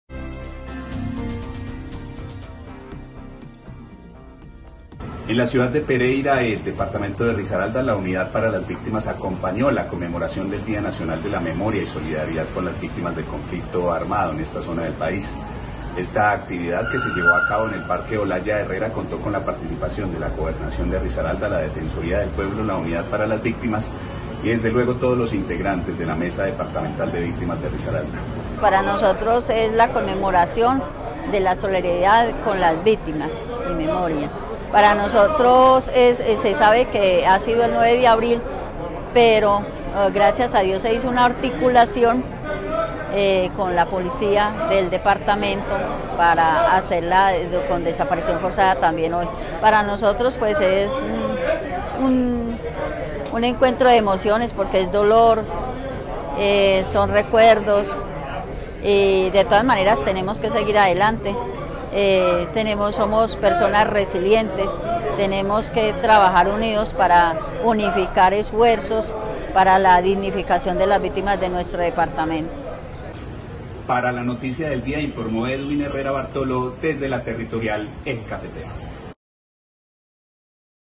📺 La Noticia del Día #152
📺 La Noticia del Día #152. @UnidadVíctimas territorial Eje cafetero participó de la conmemoración del día de las víctimas en Risaralda.